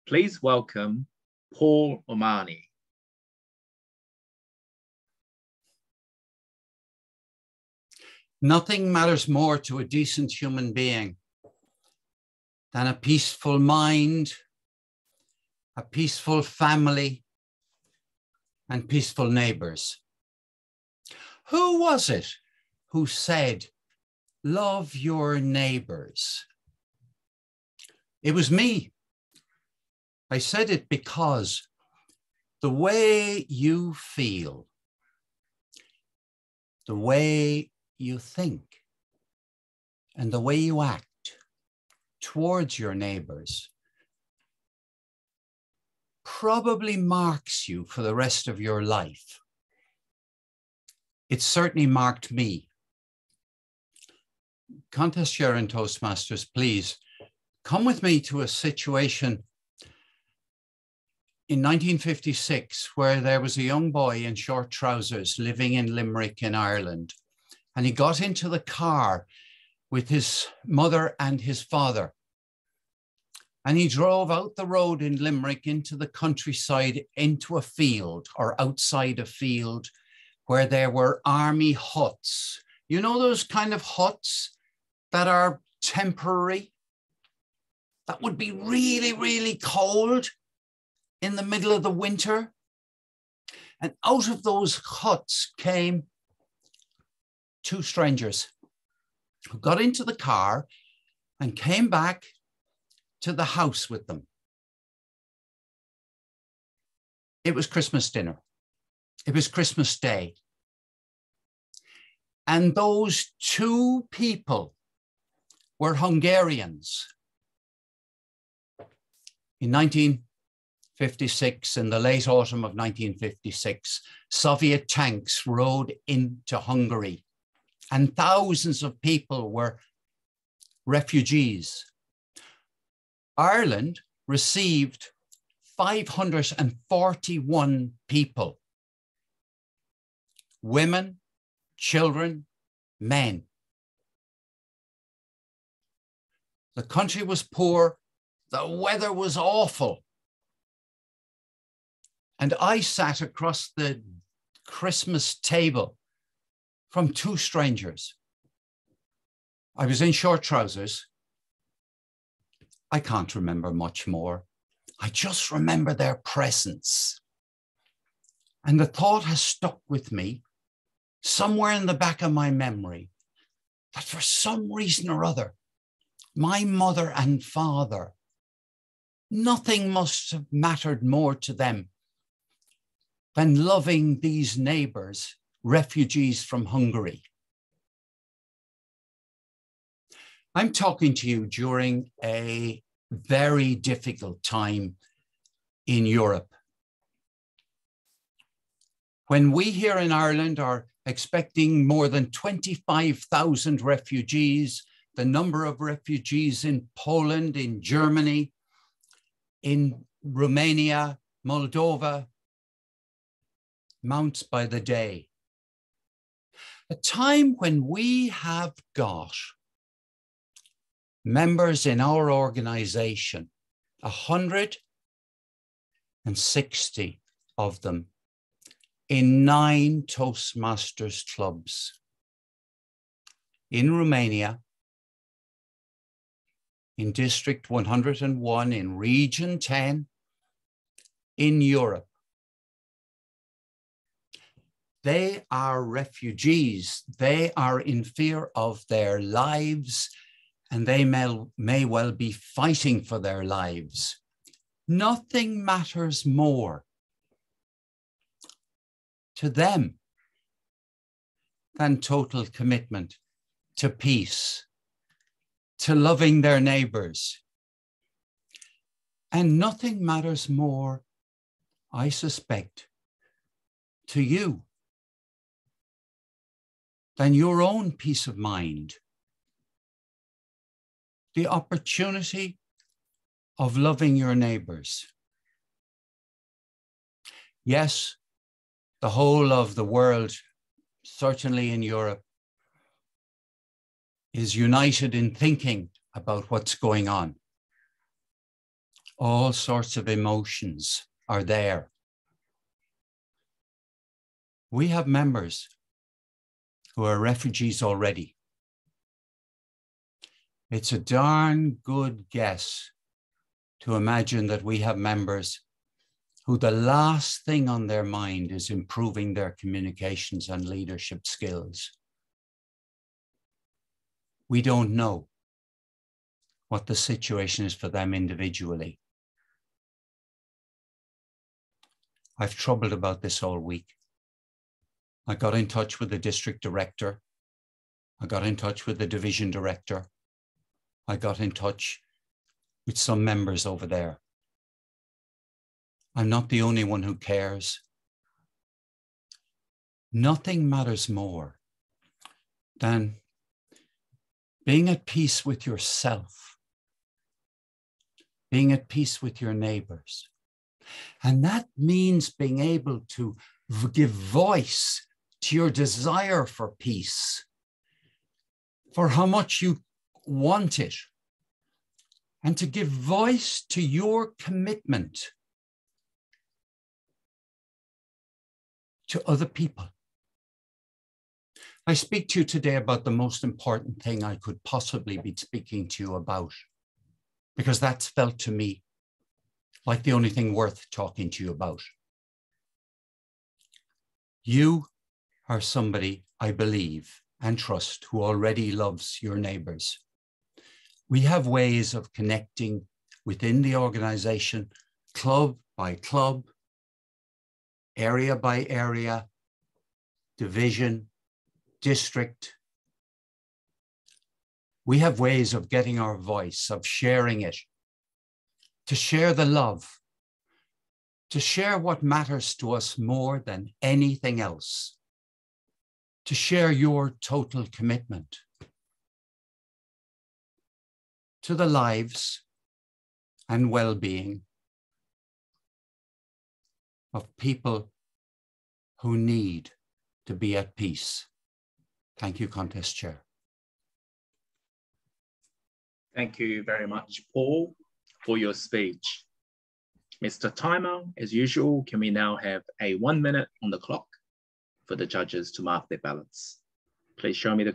Ukraine speech to Toastmasters in Firebirds Collective Club
Paul O'Mahony was a contestant in the Toastmasters International Speech Contest Final in the One Country One World Club - on Sunday 13th March 2022.  The time allowed for the speech was 5-7 minutes

The title of the speech is "NOTHING MATTERS MORE"